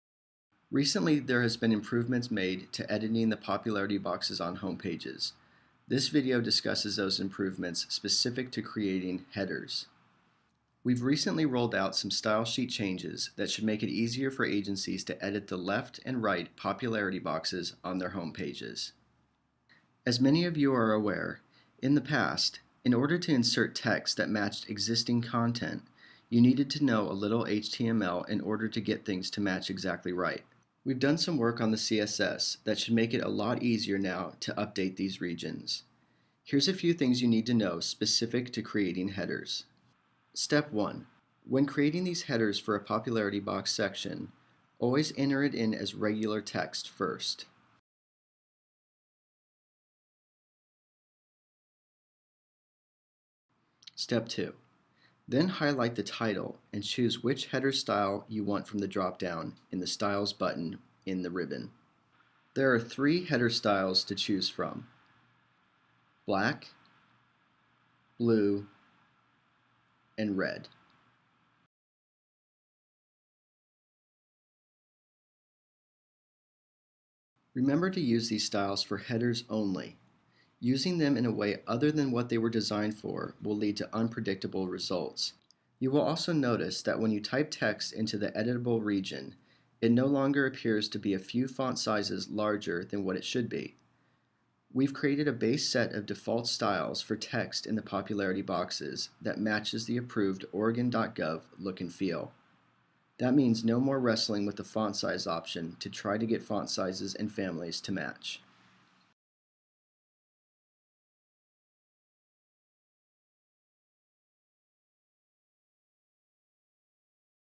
"Headers" - Improvements to Editing the Popularity Boxes on Home Pages
This video demonstrates one of the improvements when editing Popularity Boxes on agency home pages, creating Headers.